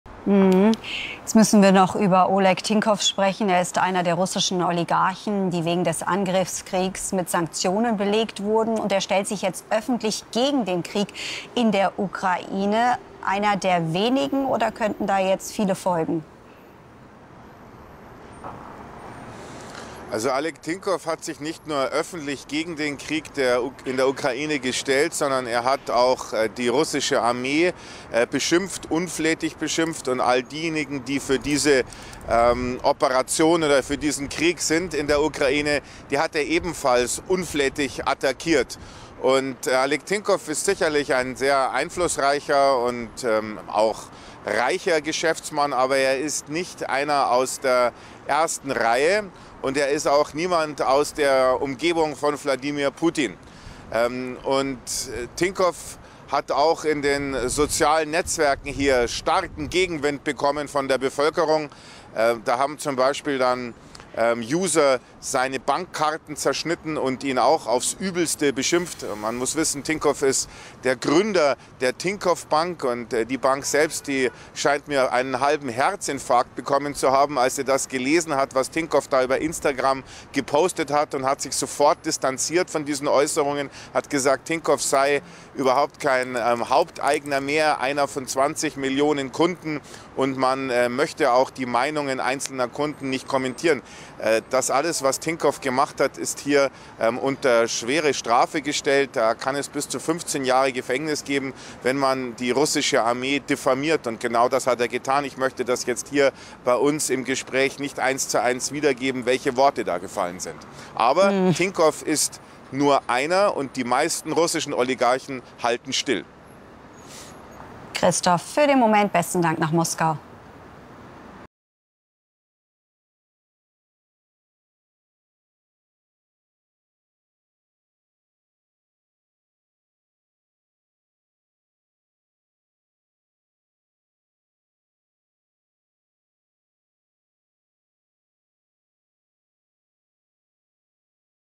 UKRAINE-KRIEG: Russischer Milliardär wütet! Russland habe eine "Scheißarmee" - "Irrsinniger Krieg!"
Deutliche Worte von Oleg Tinkow: Der russische Milliardär fordert ein Ende des „irrsinnigen Krieges“. Russland habe eine „Scheißarmee“, auch der Rest des Landes sei „beschissen“, wütet der Gründer der Tinkoff-Bank.

Der russische Milliardär Oleg Tinkow hat der russischen Armee vorgeworfen, „Massaker“ in der Ukraine zu verüben. In einer Botschaft im Onlinedienst Instagram forderte Tinkow am Dienstag ein Ende des „irrsinnigen Krieges“ gegen das Nachbarland. „90 Prozent der Russen sind gegen diesen Krieg“, schrieb der im Ausland lebende Gründer der Tinkoff-Bank. Nur eine Minderheit unterstütze den Krieg. „Aber zehn Prozent jedes Landes sind Idioten.“

Tinkow gehört zu jenen russischen Oligarchen, die wegen des Angriffskriegs gegen die Ukraine mit westlichen Sanktionen belegt wurden. In seinem Instagram-Post ging er nun hart mit der russischen Armee und dem Führungszirkel um Kreml-Chef Wladimir Putin ins Gericht.